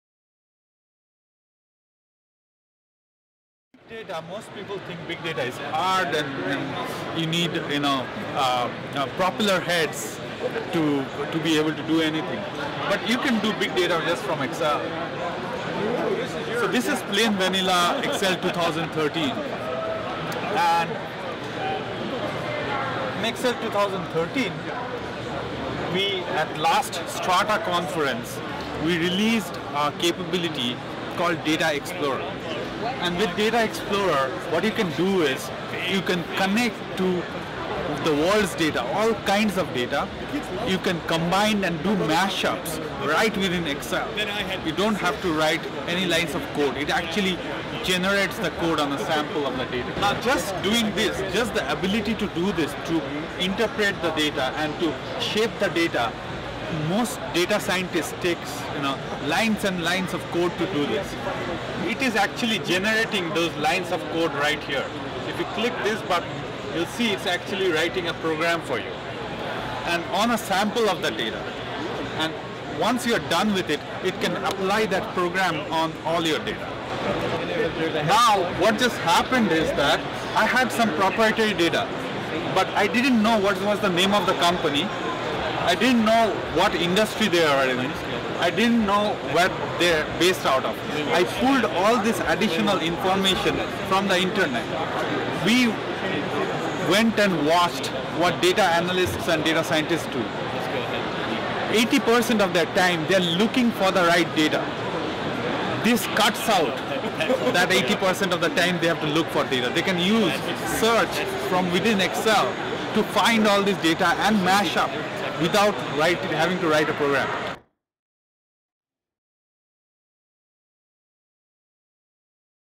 How To Become A Big Data Guru With Excel
90 percent of a data scientist's job involves finding data from a wide variety of sources and then collating the masses of information into digestible tables. Microsoft's Data Explorer is an add-in for Excel 2012 that essentially does the same job in a few mouse clicks. We were given a demonstration of how it works at TechEd 2013...